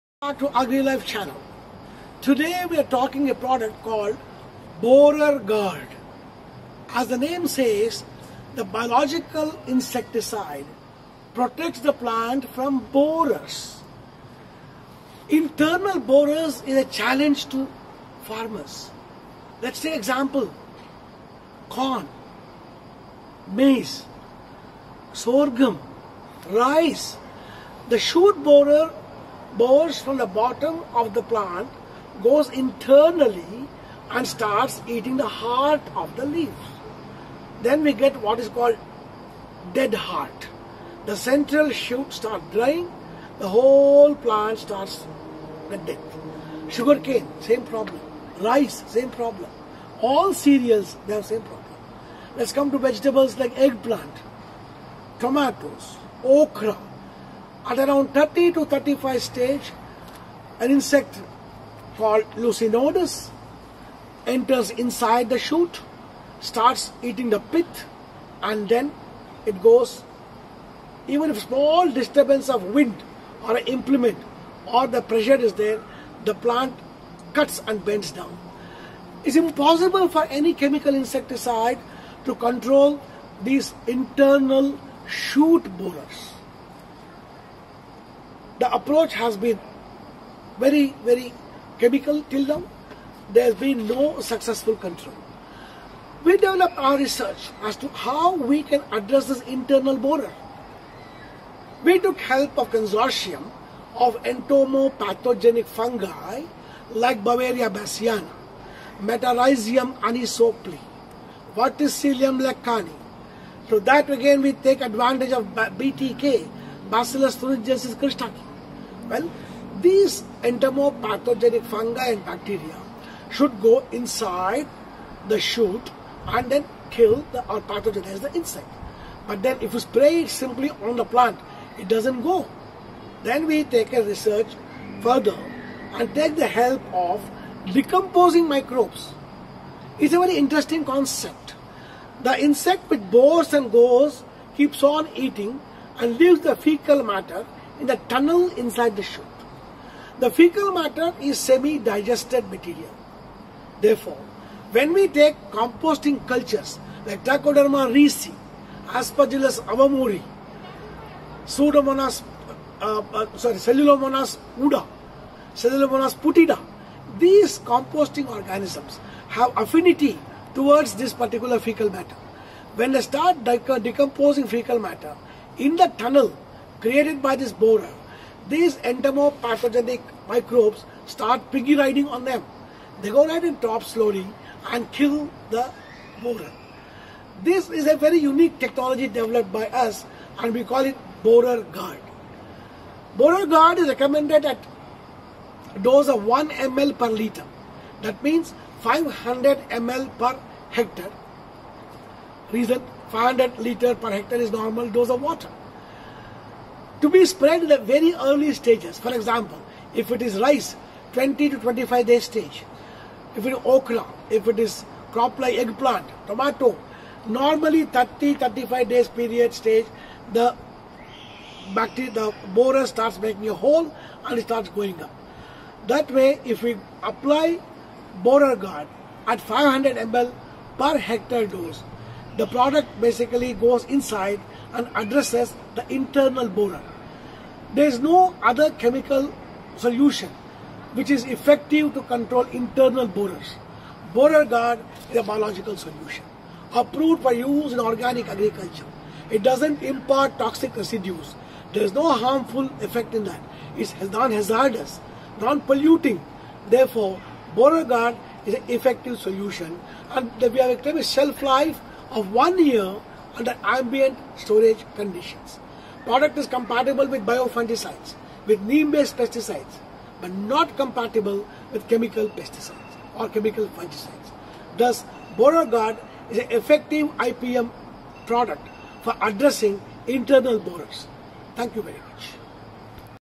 Borerguard™
Borerguard™ is a unique biological microbial consortium that controls various types of borers like stem borer and shoot borer. It contains a consortium of BioPesticide cultures ( Metarhizium anisopliae , Beauveria bassiana , Lecanicillium lecanii , Bt-K and more ) and composting cultures. The borer which enter the plant excrete faecal matter and bore internally causing a dead heart and eventually killing the plant.

Chemical pesticides do not penetrate inside the plant and therefore have little control. The composting cultures contained in Borerguard™ have an affinity to faecal matter and enter inside the plant to compost it. The BioPesticide cultures will piggy back on the composting cultures to enter and pathogenies the borers.

Crops typically affected are Brinjal (Egg Plant), Tomato, Okra (Lady’s finger / Bhendi ), Coffee, Grapes Rice, Maize, Sorghum and Sugarcane

 Borerguard™ is certified organic and is approved to use in organic agriculture

Applications: Bio Fungicide Application Types: Soil application, Foliar application, Plant swabbing

- Refer BioPesticides